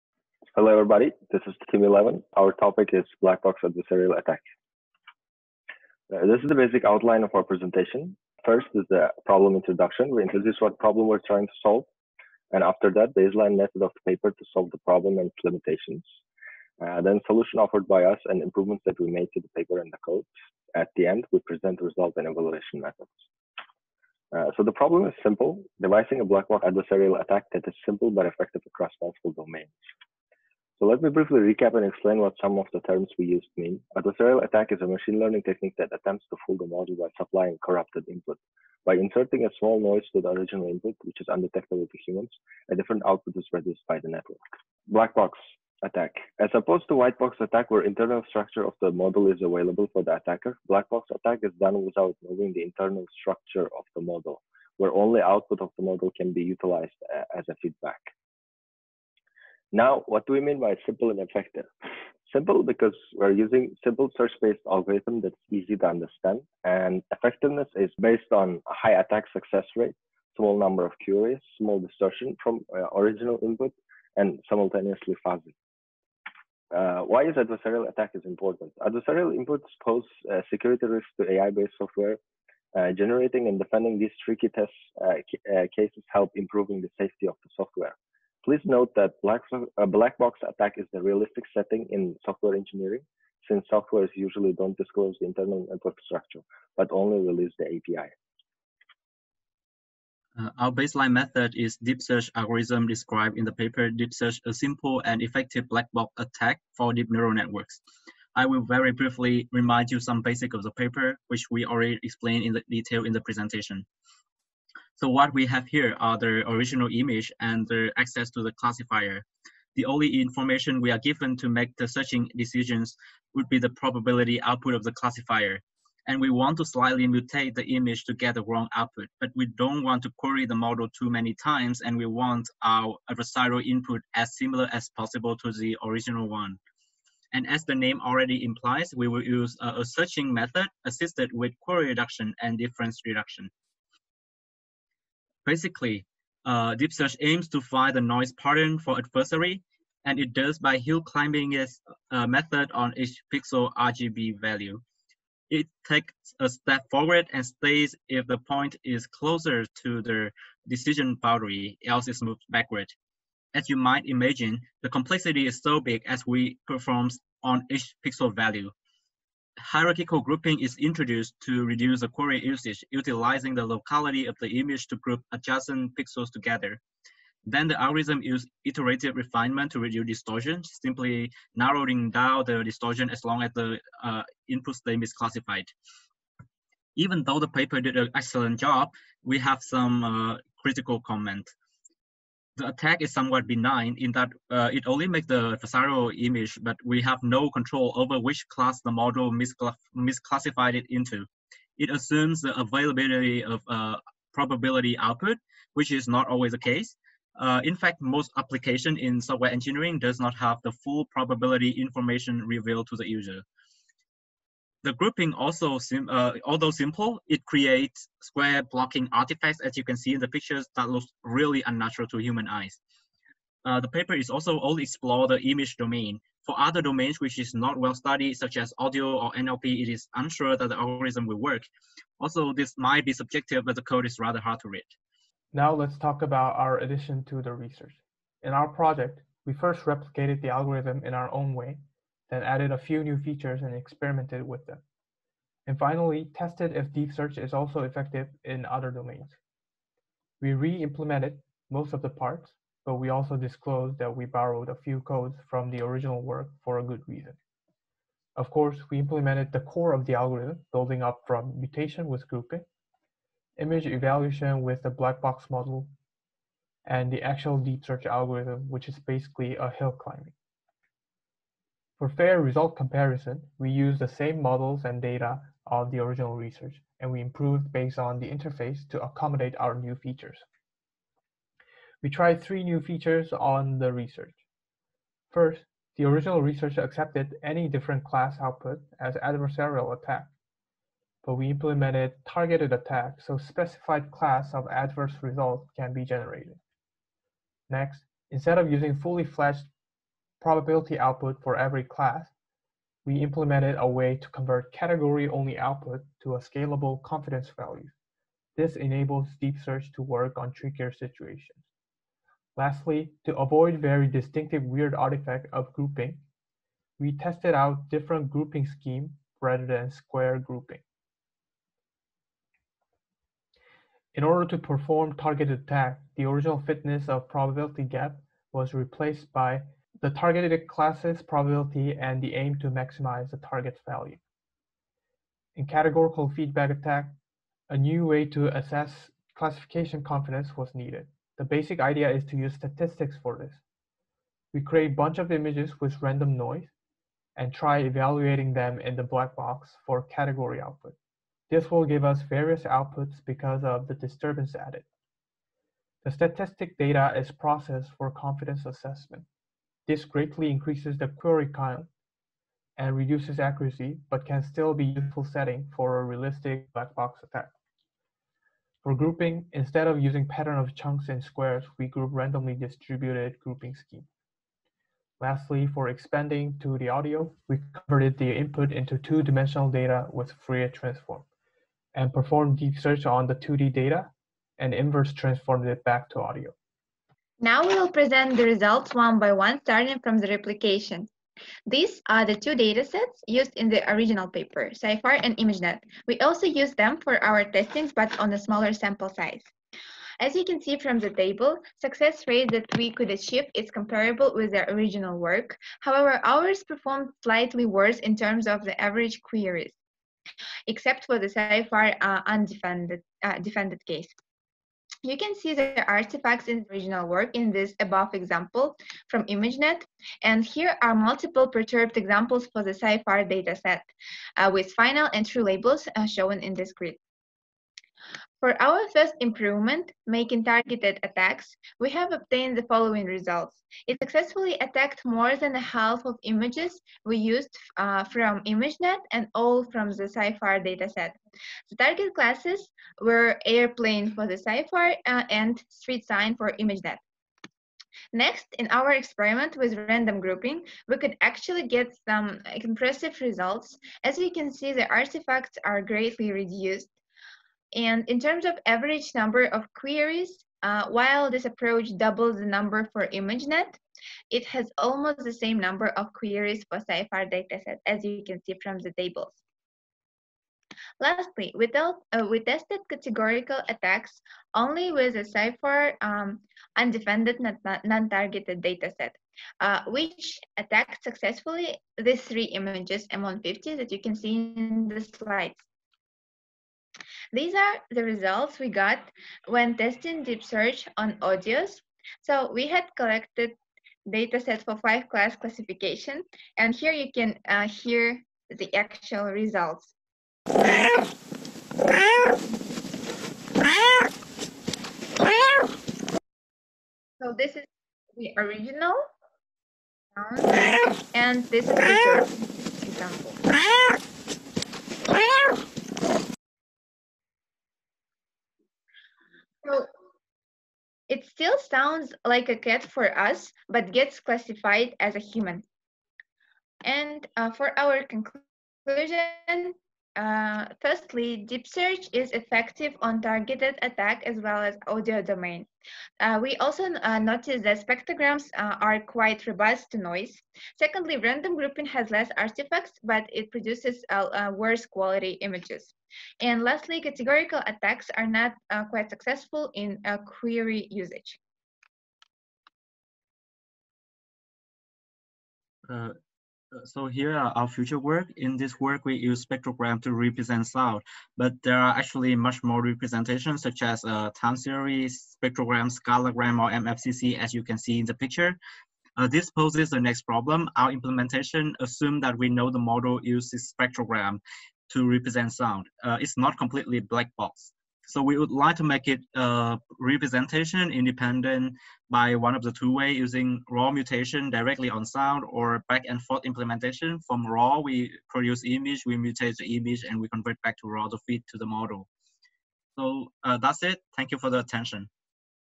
Team 11 final presentation (Improvements on DeepSearch)
2020F KAIST CS454 - AIBSE (SBSE) DeepSearch extension project.

Result images & audios: (check the log in the upper directory for class description)

The original research explored a new, simple method to fool image classifiers.
In this project, we added
-targeted attack: adversarial input can now be classified to a specific class.
-categorical output attack: probability output is not necessary, but will increase query counts.
-natural modification pattern: original researches result has distintive effect to the image. we reduced the effect.
-adversarial attack on other domain: we applied the same methodology on audio.